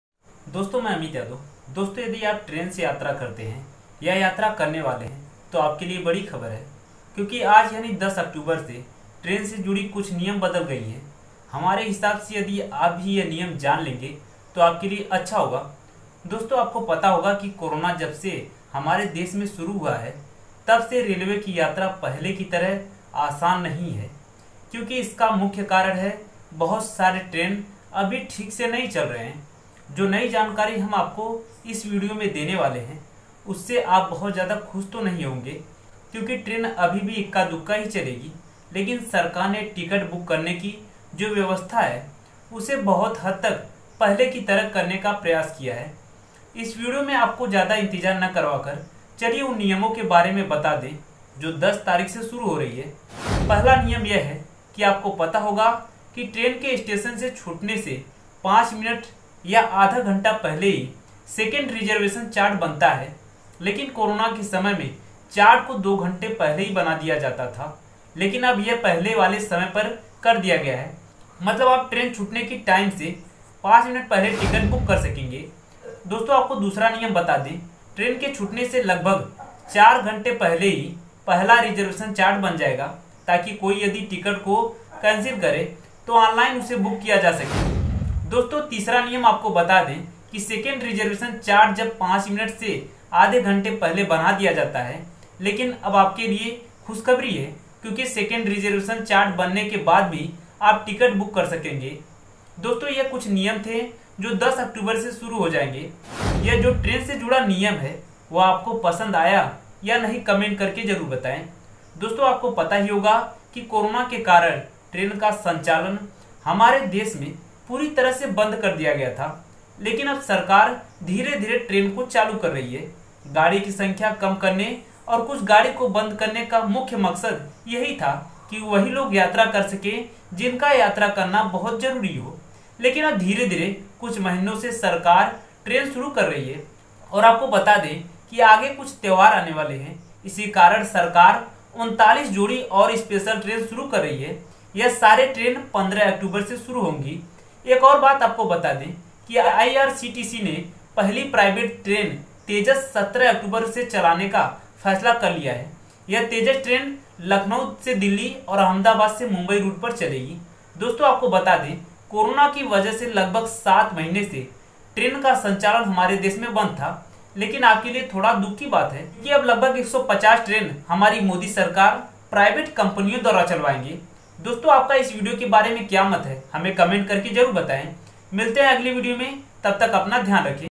आज की बड़ी खबर क्योंकि आज 10 अक्टूबर से ट्रेन की यह नियम बदल गया!train new rule 10 october today
आज की बड़ी खबर क्योंकि आज 10 अक्टूबर से ट्रेन की यह नियम बदल गया

दोस्तों मैं अमित यादव दोस्तों यदि आप ट्रेन से यात्रा करते हैं या यात्रा करने वाले हैं तो आपके लिए बड़ी खबर है क्योंकि आज यानी 10 अक्टूबर से ट्रेन से जुड़ी कुछ नियम बदल गई है हमारे हिसाब से यदि आप भी यह नियम जान लेंगे तो आपके लिए अच्छा होगा दोस्त आपको पता होगा कि कोरोना जब से हमारे देश में शुरू हुआ तब से रेलवे की यात्रा पहले की तरह आसान नहीं है क्योंकि इसका मुख्य कारण है बहुत सारे ट्रेन अभी ठीक से नहीं चल रहे हैं जो नई जानकारी हम आपको इस वीडियो में देने वाले हैं उससे आप बहुत ज्यादा खुस तो नहीं होंगे क्योंकि ट्रेन अभी भी इक्का-दुक्का ही चलेगी लेकिन सरकार ने टिकट बुक करने की जो व्यवस्था है उसे बहुत हद तक पहले की तरह करने का प्रयास किया है इस वीडियो में आपको ज्यादा इंतजार न करवाकर चलिए उन नियमों के बारे में बता दें जो 10 तारीख से शुरू हो रही है पहला नियम यह है कि आपको पता होगा कि ट्रेन के स्टेशन से छूटने से 5 मिनट से आधा घंटा पहले सेकंड रिजर्वेशन चार्ट बनता है लेकिन कोरोना के समय में चार्ट को 2 घंटे पहले ही बना दिया जाता था लेकिन अब यह पहले वाले समय पर कर दिया गया है मतलब आप ट्रेन छूटने की टाइम से 5 मिनट पहले टिकट बुक कर सकेंगे
दोस्तों आपको दूसरा नियम बता दे ट्रेन के छूटने से लगभग 4 घंटे पहले ही पहला रिजर्वेशन चार्ट बन जाएगा ताकि कोई यदि टिकट को कैंसल करें तो ऑनलाइन उसे बुक किया जा सके दोस्तों तीसरा नियम आपको बता दें कि सेकंड रिजर्वेशन चार्ट जब 5 मिनट से आधे घंटे पहले ही बना दिया जाता है लेकिन अब आपके लिए खुशखबरी है क्योंकि  सेकंड रिजर्वेशन चार्ट बनने के बाद भी आप टिकट बुक कर सकते हैं दोस्तों यह कुछ नियम थे जो 10 अक्टूबर से शुरू हो जाएंगे यह जो ट्रेन से जुड़ा नियम है वह आपको पसंद आया या नहीं कमेंट करके जरूर बताएं दोस्तों आपको पता ही होगा कि कोरोना के कारण ट्रेन का संचालन हमारे देश में पूरी तरह से बंद कर दिया गया था लेकिन अब सरकार धीरे-धीरे ट्रेनों को चालू कर रही है गाड़ी की संख्या कम करने और कुछ गाड़ी को बंद करने का मुख्य मकसद यही था कि  वही लोग यात्रा कर सके जिनका यात्रा करना बहुत जरूरी हो लेकिन अब धीरे-धीरे कुछ महीनों से सरकार ट्रेन शुरू कर रही है और आपको बता दें कि आगे कुछ त्यौहार आने वाले हैं इसी कारण सरकार 39 जोड़ी और स्पेशल ट्रेन शुरू कर रही है यह सारे ट्रेन 15 अक्टूबर से शुरू होगी एक और बात आपको बता दें आईआरसीटीसी ने पहली प्राइवेट ट्रेन तेजस 17 अक्टूबर से चलाने का फैसला कर लिया है यह तेजस ट्रेन लखनऊ दिल्ली और अहमदाबाद मुंबई रूट पर चलेगी दोस्तों आपको बता दें कोरोना की वजह से लगभग 7 महीने से ट्रेन का संचालन हमारे देश में बंद था लेकिन आपके लिए थोड़ा दुख की बात है क्योंकि अब लगभग 150 ट्रैन हमारी मोदी सरकार प्राइवेट कंपनियों द्वारा  चलवाएंगे दोस्त आपका इस वीडियो के बारे में क्या मत है हमें कमेंट करके जरूर बताएं मिलते हैं अगली वीडियो में तब तक अपना ध्यान रखे